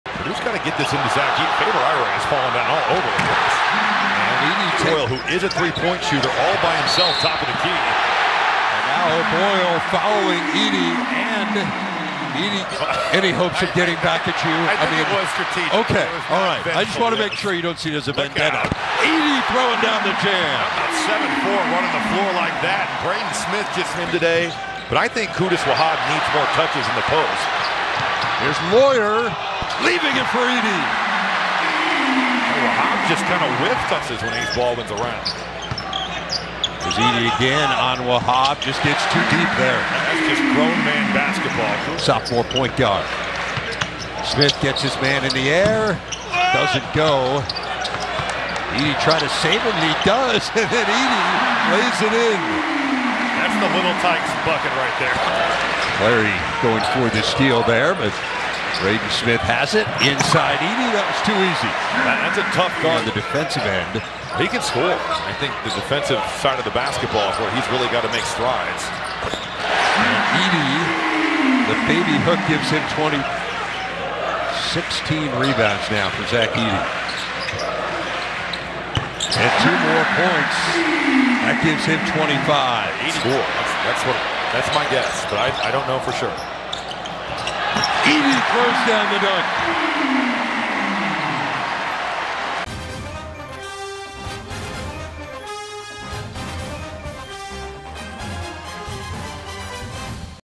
Zach Edey DOMINATES with 30 points and 20 rebounds in Purdue’s win over Penn State | CBB on FOX
Zach Edey dominated with 30 points and 20 rebounds in the Purdue Boilermakers' 95-78 win over the Penn State Nittany Lions.